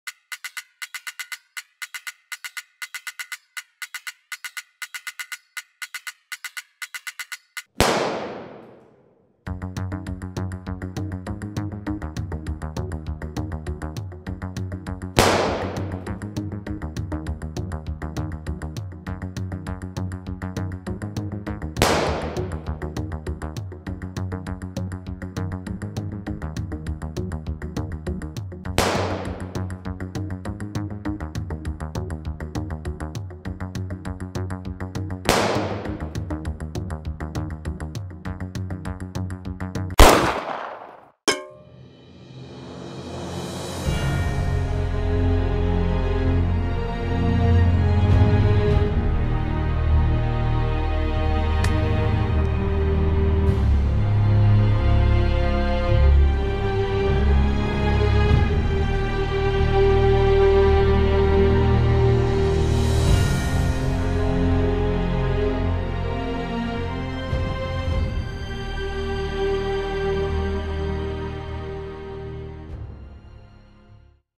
Hi-Point 995TS carbine in action against a beer can
Using a 9mm carbine set up with Holosun red dot, Angel Kim vanquishes the tin can once and for all.